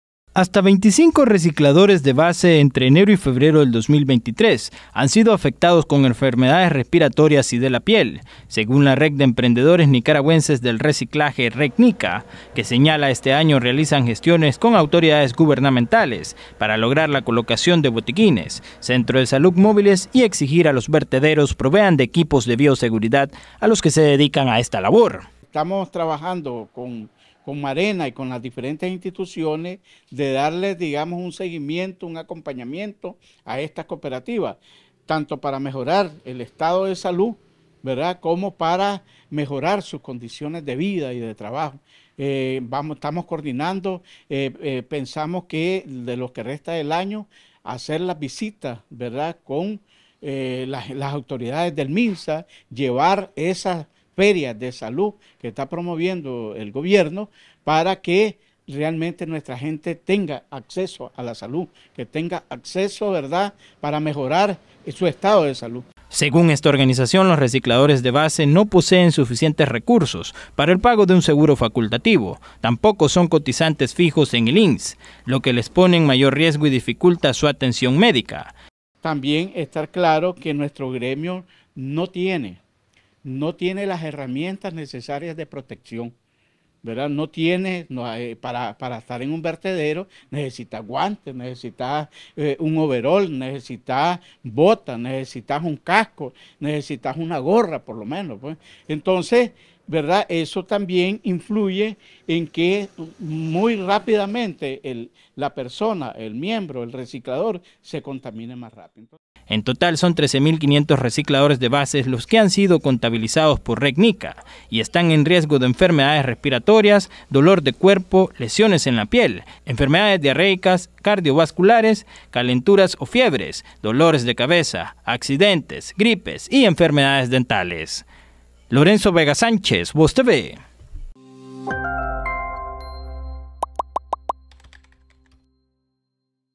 Recicladores de base en Nicaragua expuestos a varias enfermedades
Al menos 25 recicladores de base, entre enero y febrero del 2023, han sido afectados con enfermedades respiratorias y de la piel, según la Red de Emprendedores Nicaragüenses del Reciclaje (Rednica), que señala este año realizan gestiones con autoridades gubernamentales para lograr la colocación de botiquines, centros de salud móviles y equipos de bioseguridad a los que se dedican a esta labor en los vertederos.

David Narváez, presidente de Rednica, indican que están trabajando junto a Marena y con diferentes instituciones gubernamentales para darle un seguimiento a estas cooperativas.